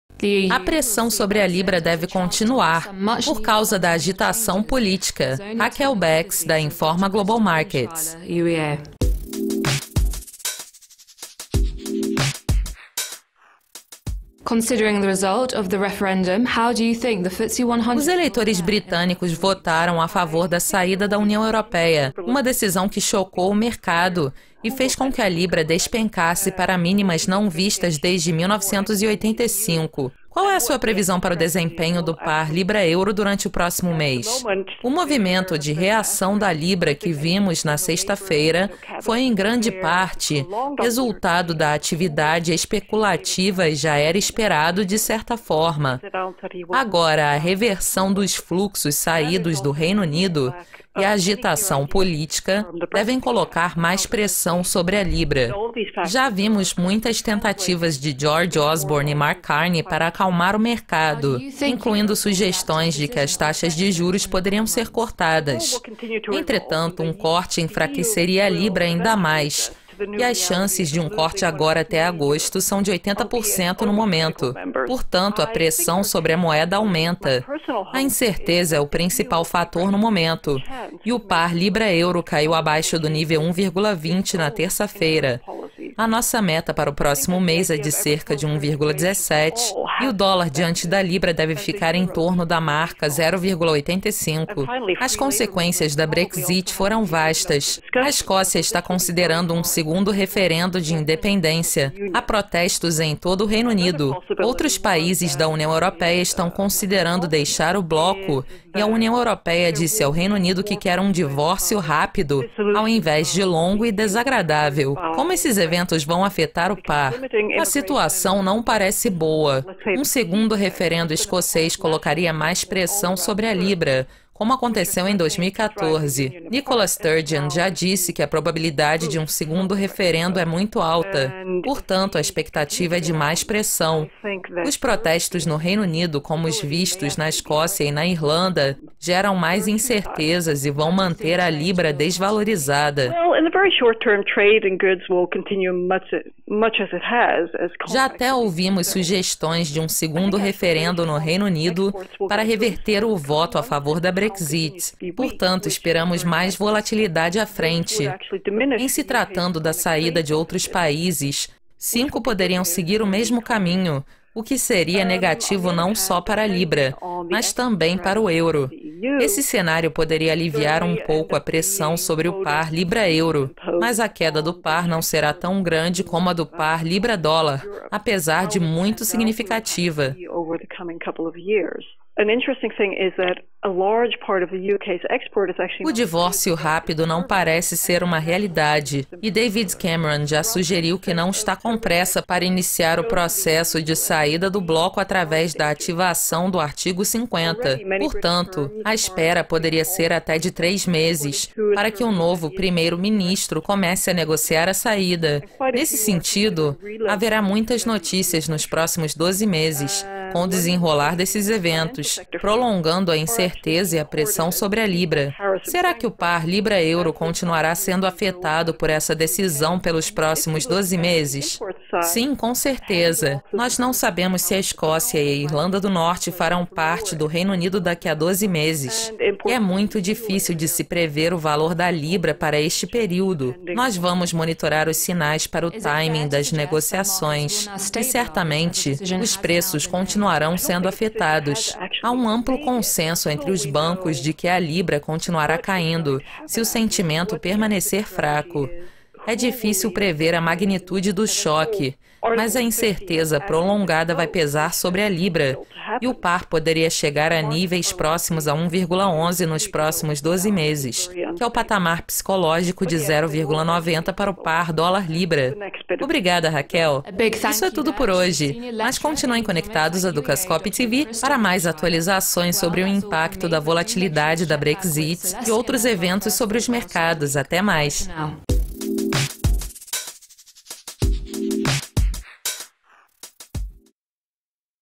Tempo de reflexão para a UE
As exportações do Reino Unido poderiam ser estimuladas nos próximos meses, mas a União Europeia deve reavaliar suas políticas internas. Dr Christa Brunnschweiler, UEA.